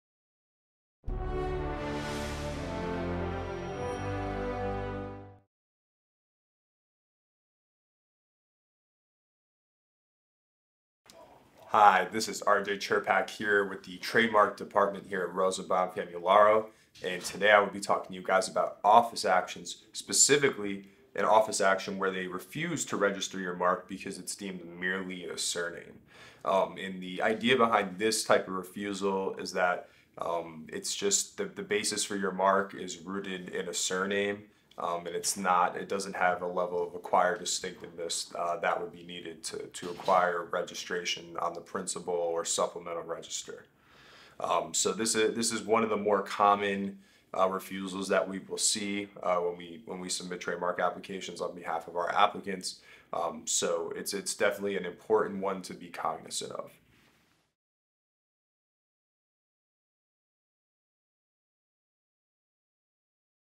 Trademark Series: Discusses Merely A Surname Office Actions
Office actions, specifically an office action where they refuse to register your mark because it's deemed merely asserting. And the idea behind this type of refusal is that it's just the basis for your mark is rooted in a surname and it doesn't have the level of acquired distinctiveness that would be needed to acquire registration on the principal or supplemental register. So this is one of the more common refusals that we will see when we submit trademark applications on behalf of our applicants. So it's definitely an important one to be cognizant of.